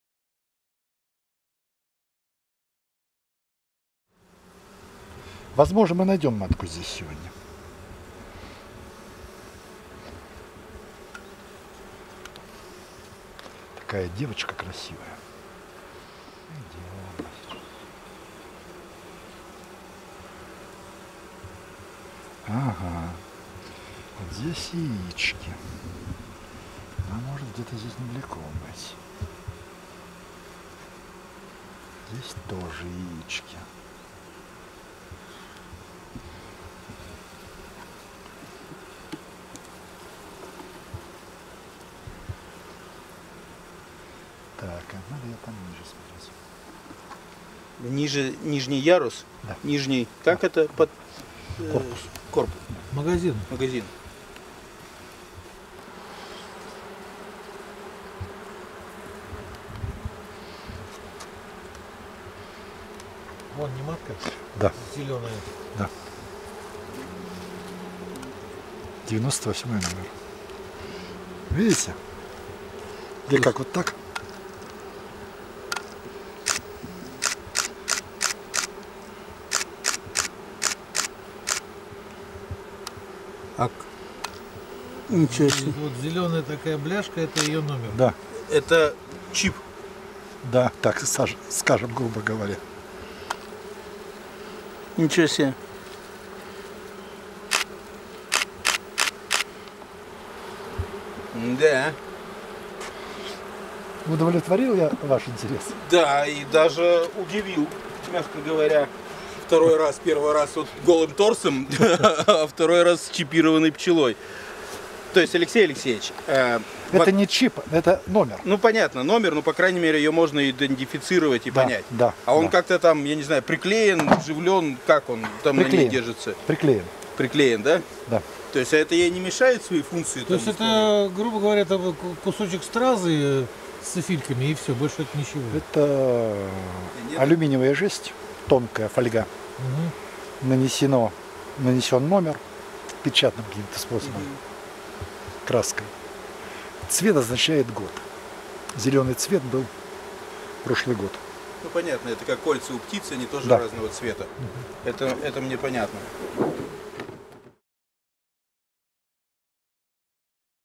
Чипированная пчела. Хозяйство А.А Цветкова
Чипированная пчела. Хозяйство А.А Цветкова. Тверская область. 26 июня 2015 г. Обсуждение, много фото и другое видео